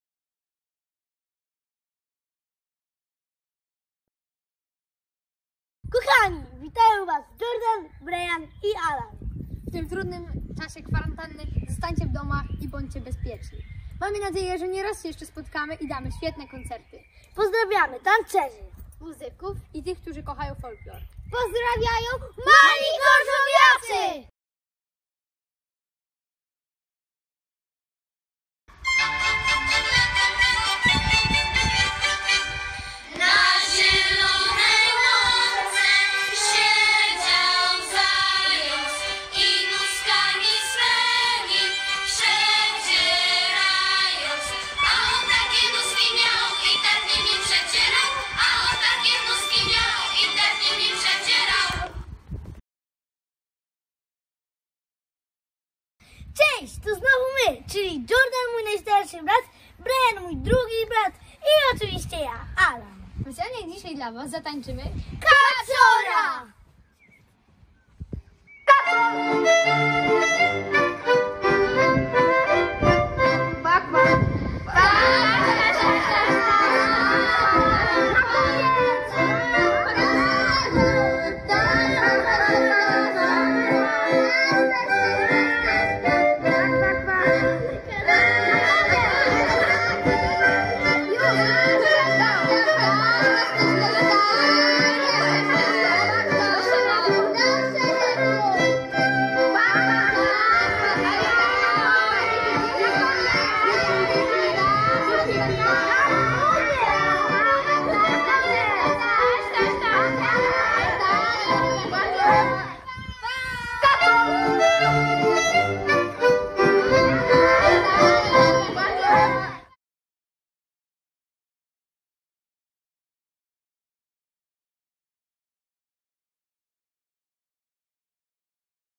Onlinowa Folk Akademia EPILOG 1
Projekt Onlinowa Folk Akademia przygotowujemy w czasie, kiedy musimy pozostawać w domowej izolacji, spowodowanej epidemią korona wirusa- wiosna 2020. W realizacji wykorzystujemy elementy polskich tańców i zabaw ludowych oraz tańców narodowych.
W tym odcinku materiał przygotowali i przedstawiają Jordan, Brajan i Alan z ZTL "Mali Gorzowiacy"

Maria Szupiluk i Zespół Tańca Ludowego "Mali Gorzowiacy", Młodzieżowy Dom Kultury w Gorzowie Wielkopolskim